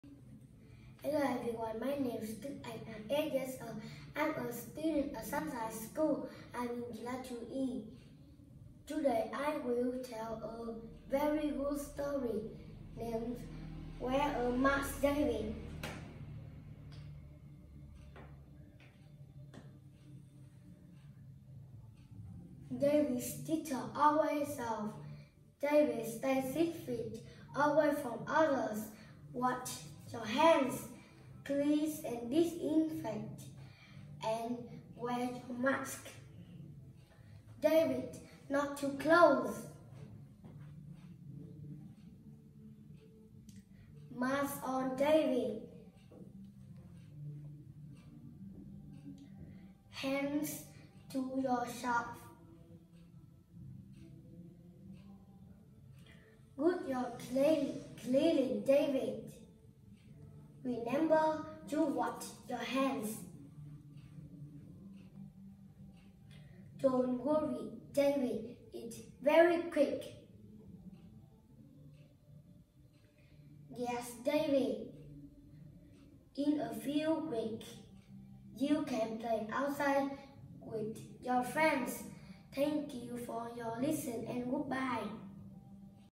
Lã Đức Anh_Sprout 2E Sunshine School: Wear a mask, David
Đức Anh rất thông minh, chăm chỉ học và đọc truyện, động tác rất đúng và biểu cảm, PA hay, giọng to rõ ràng. bố mẹ và các con cùng cổ vũ, bình chọn cho Đức Anh nhé!